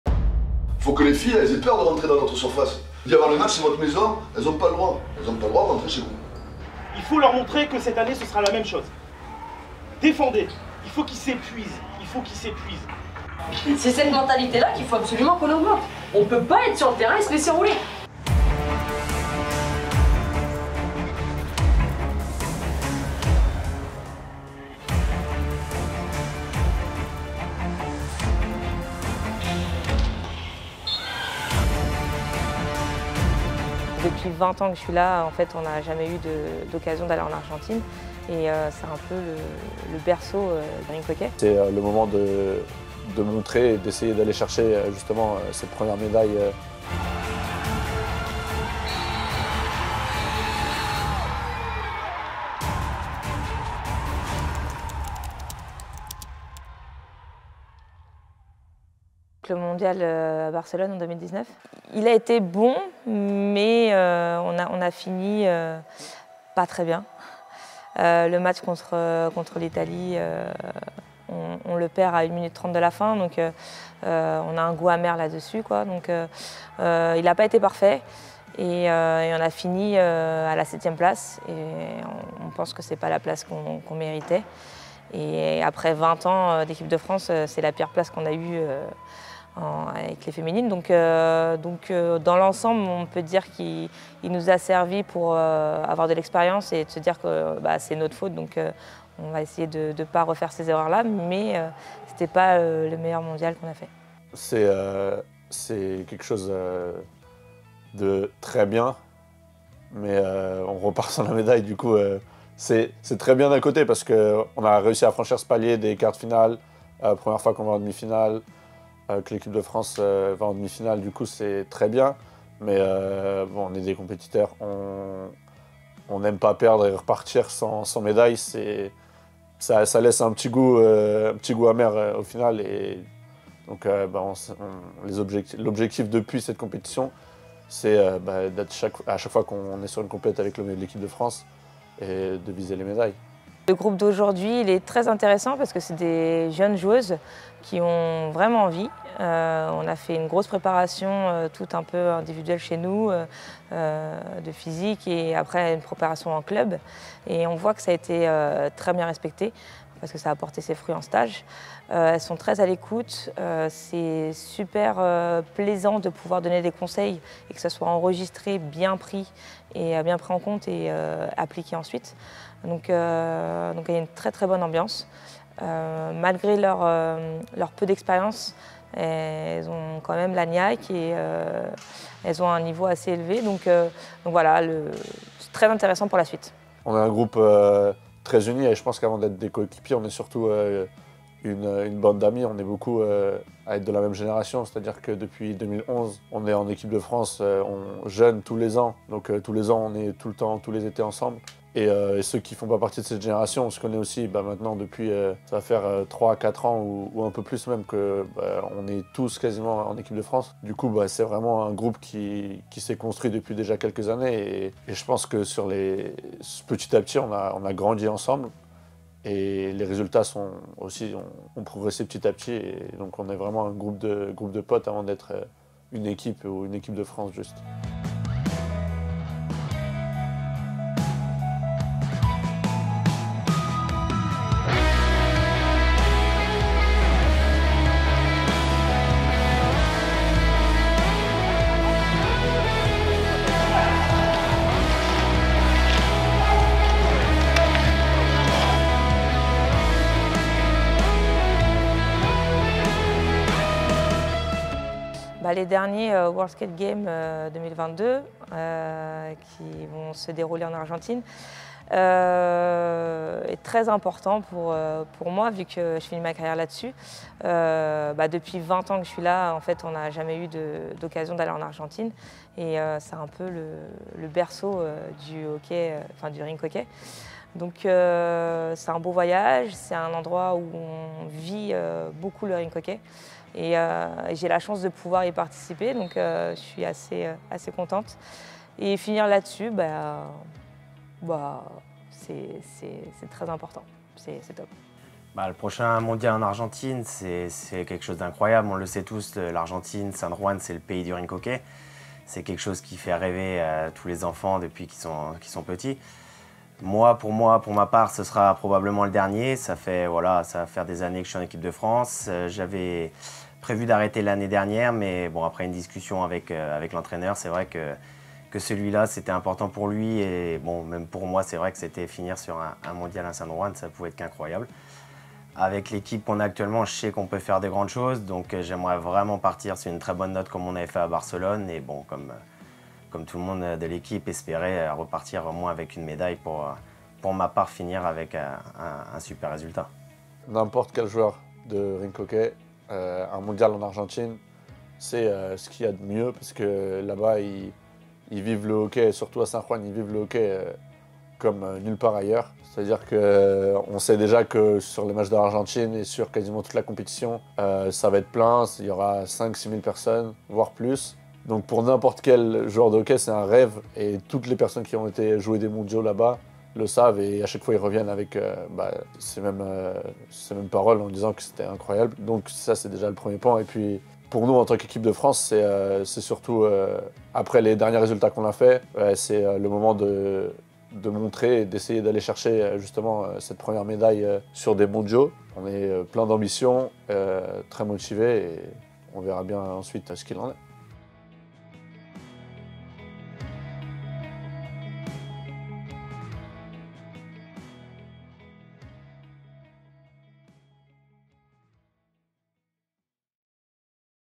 WSG 2022 "Rink hockey France - Road to Argentina"
Nos équipes de France féminine et masculine se rendront à San Juan pour participer aux World Skate Games 2022. D'avis de tous, c'est un aboutissement pour un athlète de rink hockey d'avoir un jour l'occasion de jouer dans cette ville où on voue un véritable culte à ce sport. Des tribunes déjà annoncées pleines à craquer, une salle mythique, tous les ingrédients seront réunis pour un moment qui s'annonce déjà unique !

A San Juan, on vit, on dort et on respire rink hockey...rendez-vous en terre sainte avec nos tricolores !